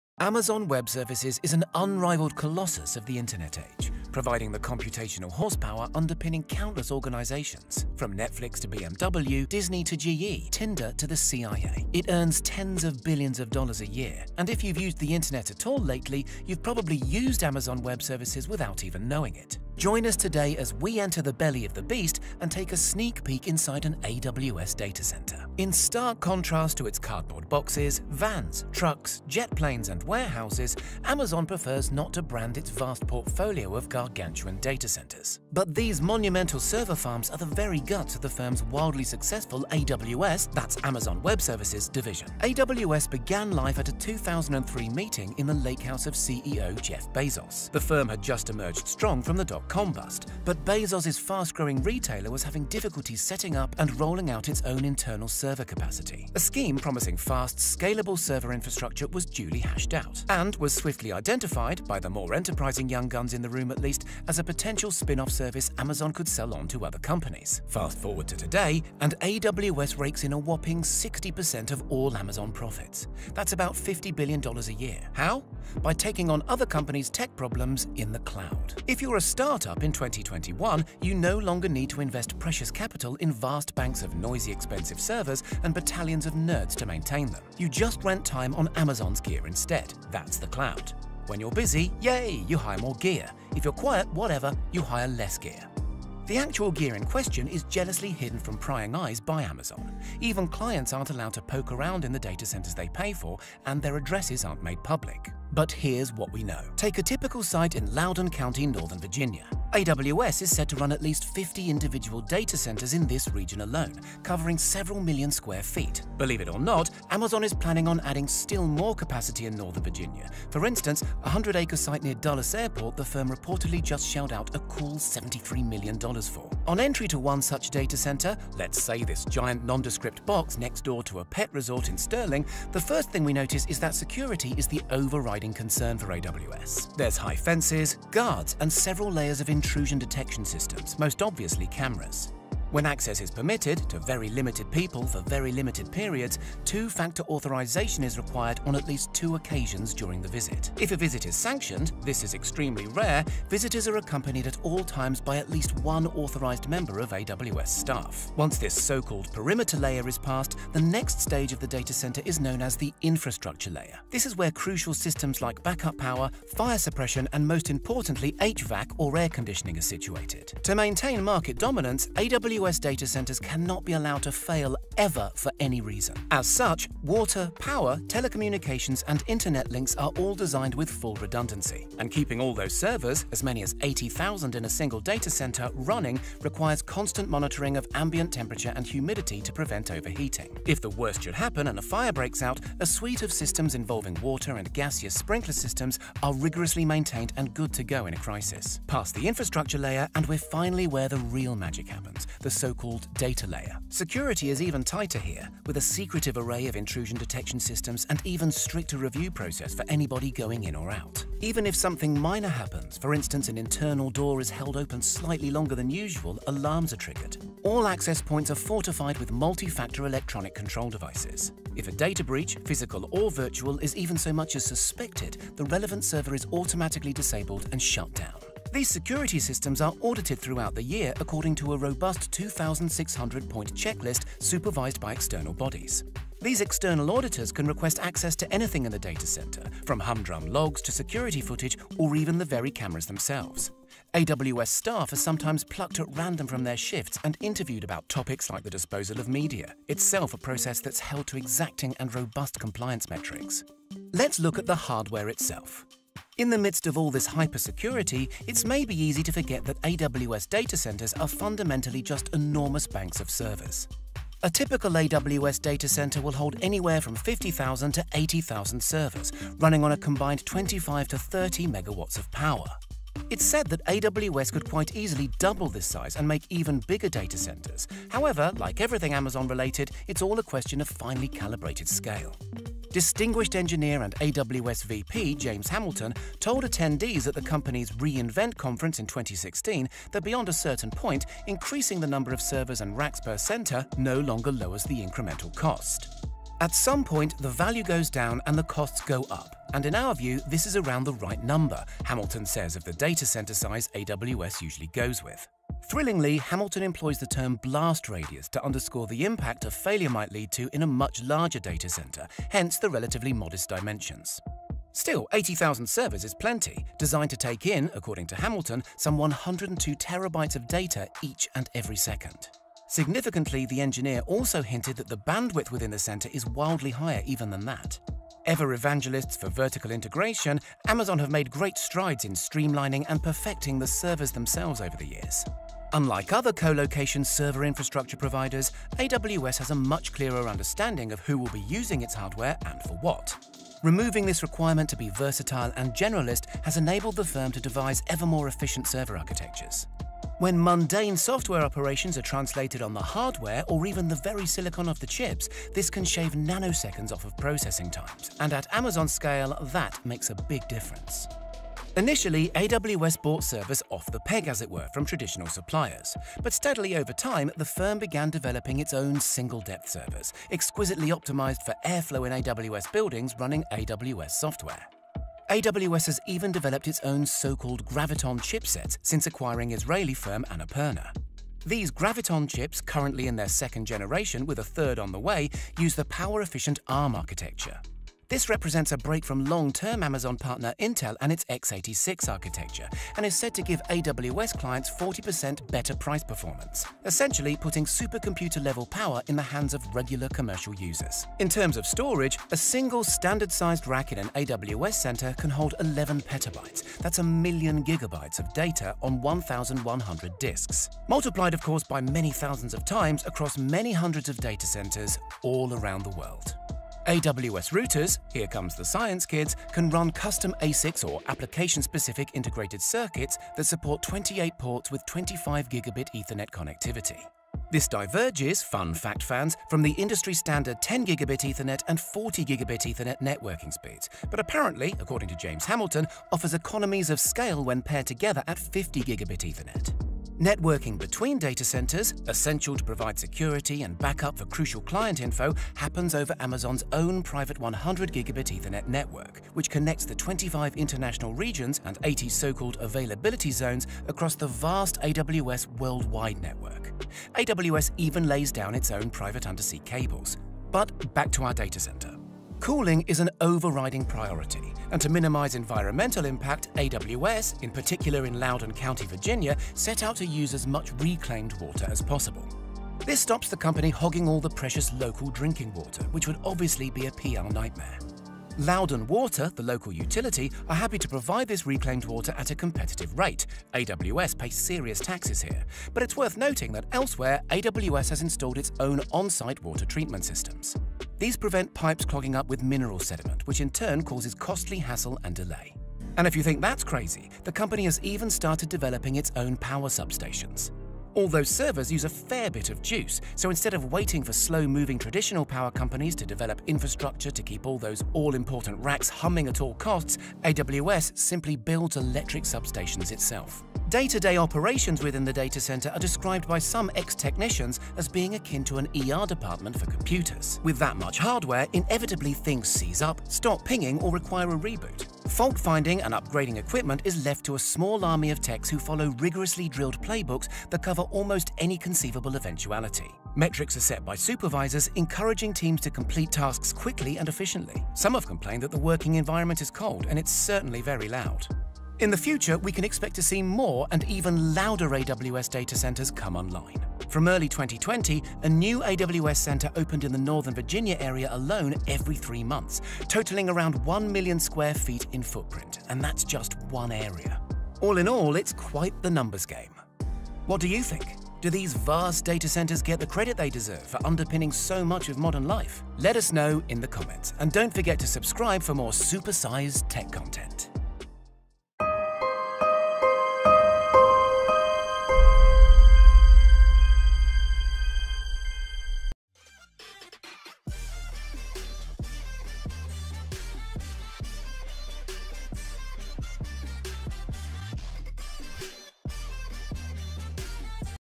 Inside Amazon's Massive Data Center
Amazon Web Services is an unrivalled colossus of the internet age, providing the computational horsepower underpinning countless organizations from Netflix to BMW, Disney to GE, Tinder to the CIA. It earns tens of billions of dollars a year, and if you’ve used the internet at all lately, you’ve probably used Amazon Web Services without even knowing it.

But where is it? What is it? And how does it all work? Join us now as we enter the belly of the beast and take a sneak peek inside an AWS data centre.

Imagery supplied via Pixel Images